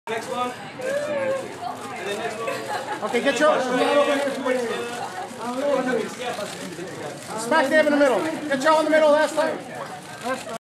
Arlenis Sosa, Lindsay Ellingson, Doutzen Kroes, Behati P...
Arlenis Sosa, Lindsay Ellingson, Doutzen Kroes,  Behati Prinsloo and Erin Heatherton at Victoria Secret at  Victoria Secret 2nd Annual Supermodel Cycle Benefiting Cancer Research at SoulCycle, Upper East Side on July 11, 2012 in New York, New York